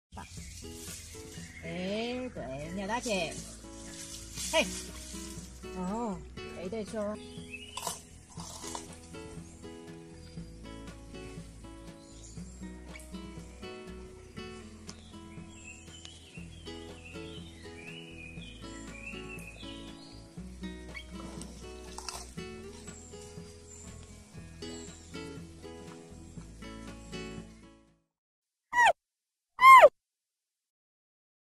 Turn Your Sounds On! Panda Xiang Xiang Enjoys Her Big Bamboo Shoots | iPanda
Video credit: China Conservation and Research Centre for the Giant Panda